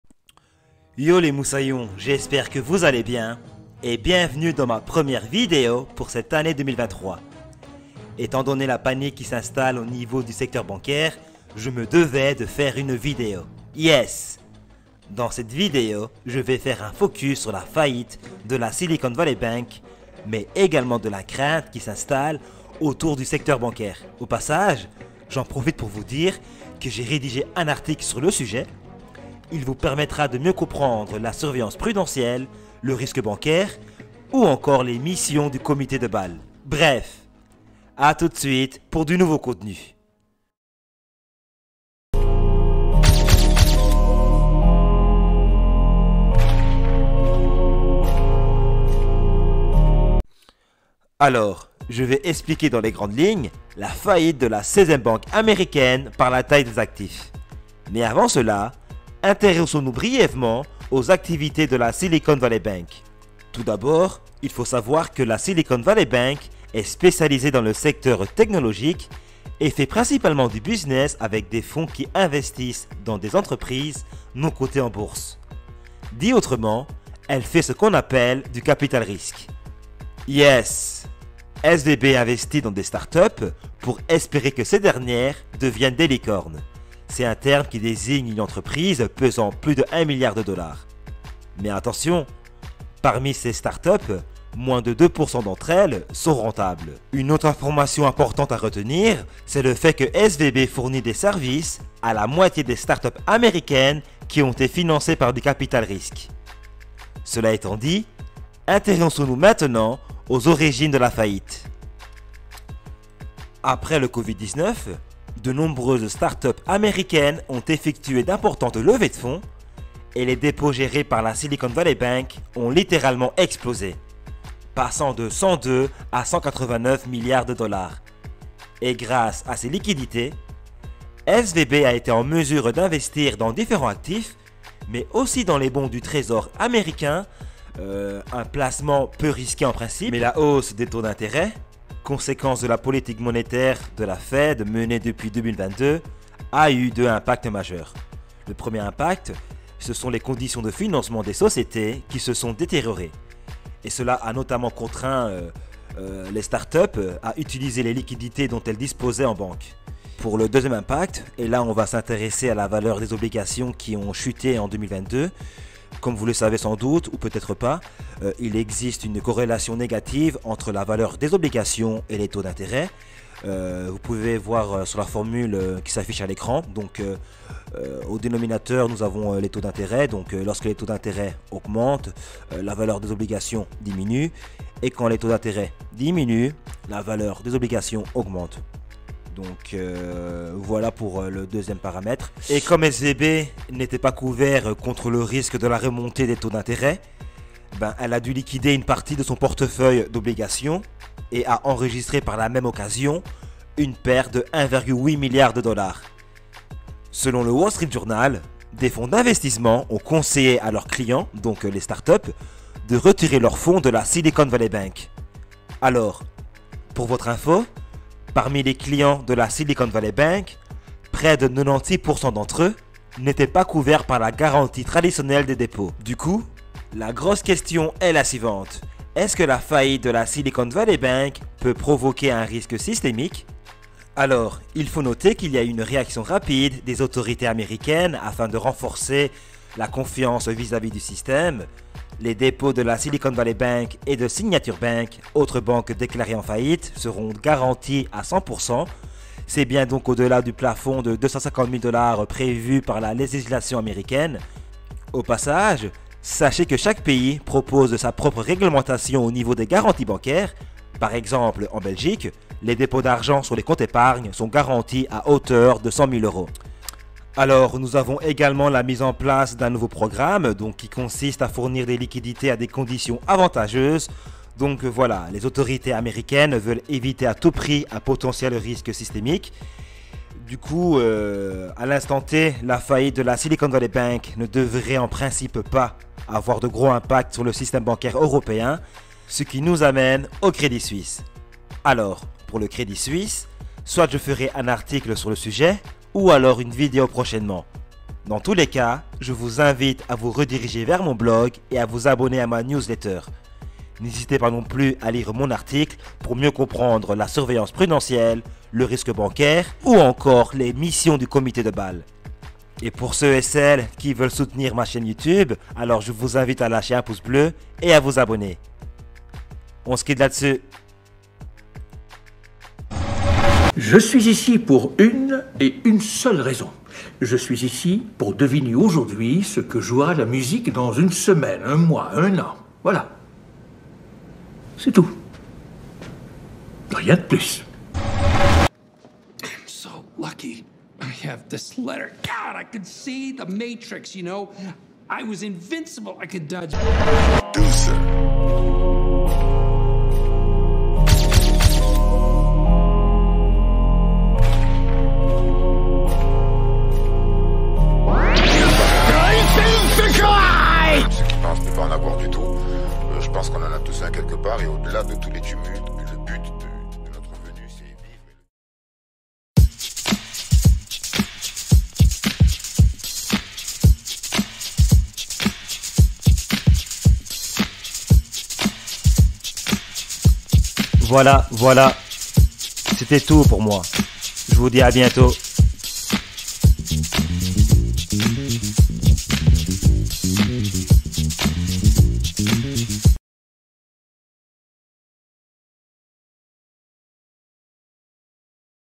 Comprendre la faillite de la Silicon Valley Bank + Article (surveillance prudentielle)
Dans cette vidéo, je vous propose de faire un focus sur les raisons de la faillite de la Silicon Valley Bank.

0:00 • Introduction (Article Surveillance Prudentielle)
0:49 • Silicon Valley Bank en quelques mots
1:51 • SVB : origines de la faillite (obligations, taux intérêts)
3:46 • Vers un Risque Systémique ?
5:23 • Fin (Fan Engagement)